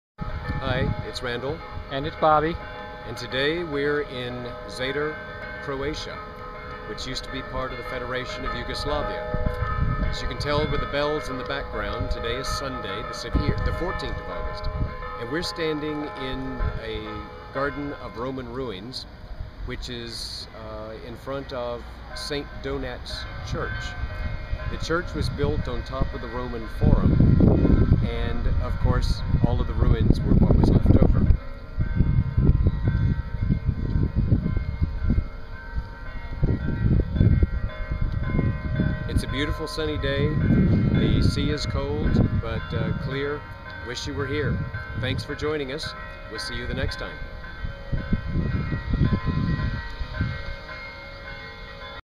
Zadar Saint Donat.mp4
Visit to Zadar Croatia with Bobby and Randell.
A view of Saint Donat's Square.